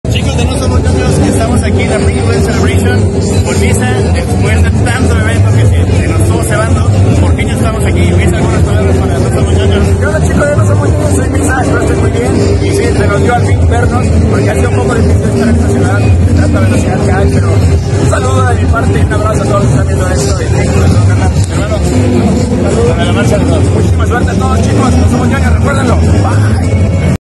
¡Nos encontramos con MissaSinfonia!
Y nos encontramos a MissaSinfonia en el Pringles Games Celebration